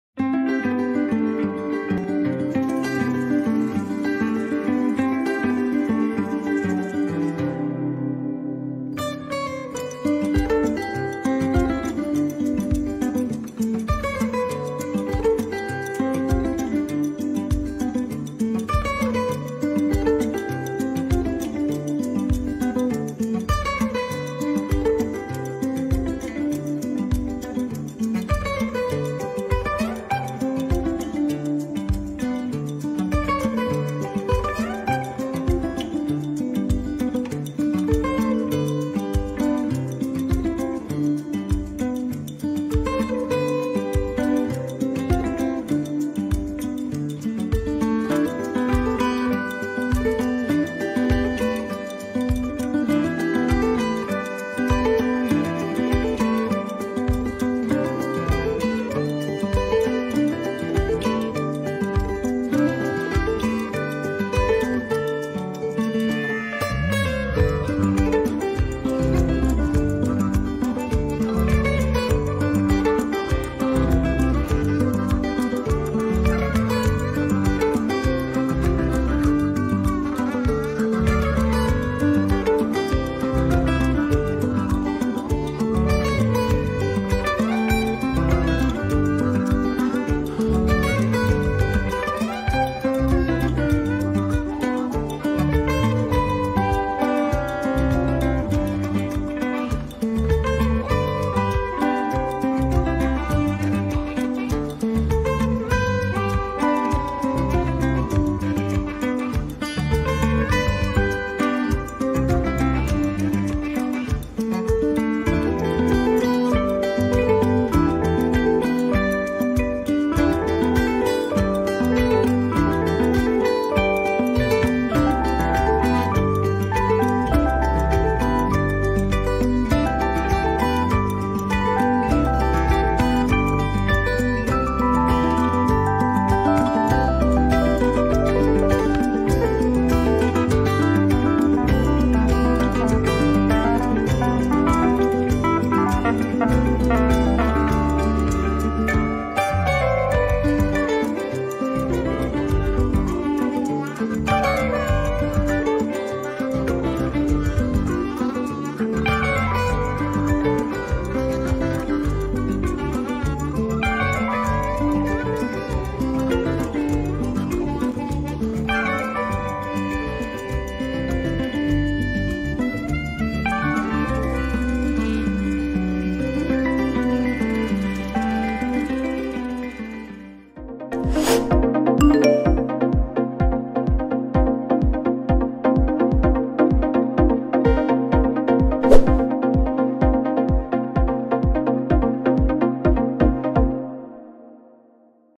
Tiene cero estrellas: el hotel que es al aire libre y que sólo tiene una cama
Null Stern es un hotel “cero estrellas”, no tiene paredes, ni ventanas, mucho menos un baño cerca, solo una cama y dos muebles ubicados a sus costados. Pero a pesar de que no hay muchas comodidades, el precio de pasar una noche en esa cama es una verdadera fortuna.